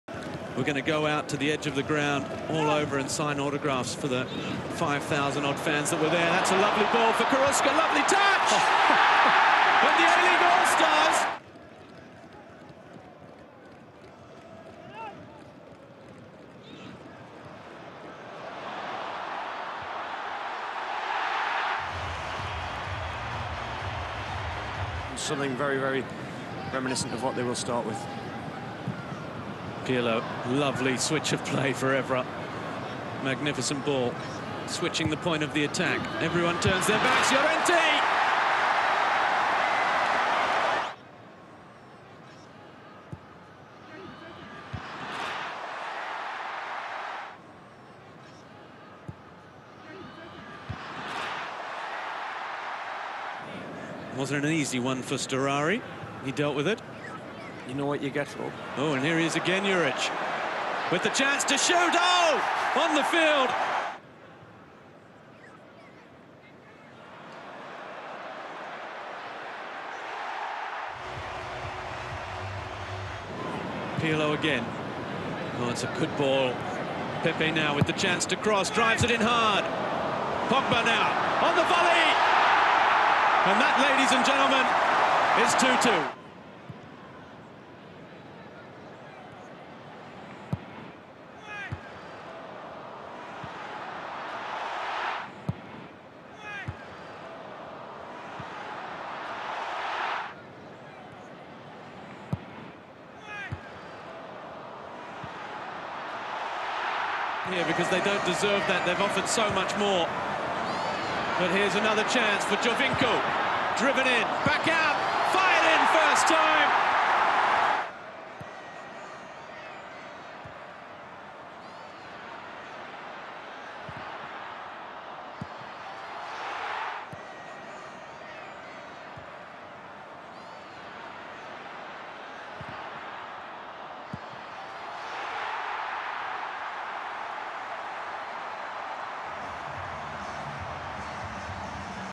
A-League All Stars - Juventus 2-3 HIGHLIGHTS
Con i gol di Llorente, Pogba e Pepe, all'ANZ Stadium di Sydney la Juventus batte per 3 a 2 le All Stars della A-League australiana capitanati da Alessandro Del Piero.

Juventus defeat the A-League Allstars, captained by Alessandro Del Piero, 3-2 at ANZ stadium thanks to goals from Fernando Llorente, Paul Pogba and Simone Pepe.